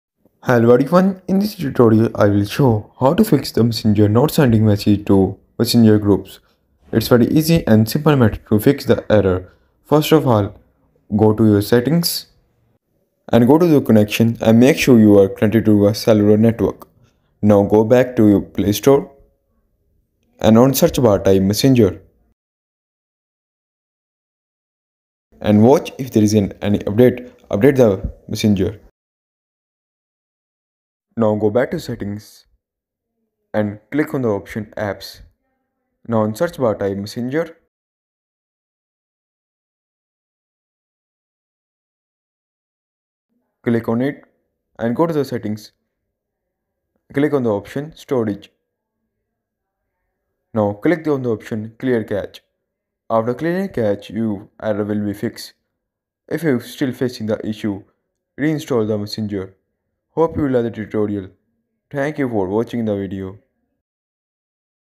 How To Fix Messenger Not Sending Messages on Group Chat
messenger facebookmessenger facebookgroup Learn how to fix messenger problem of not sending messages in group chats.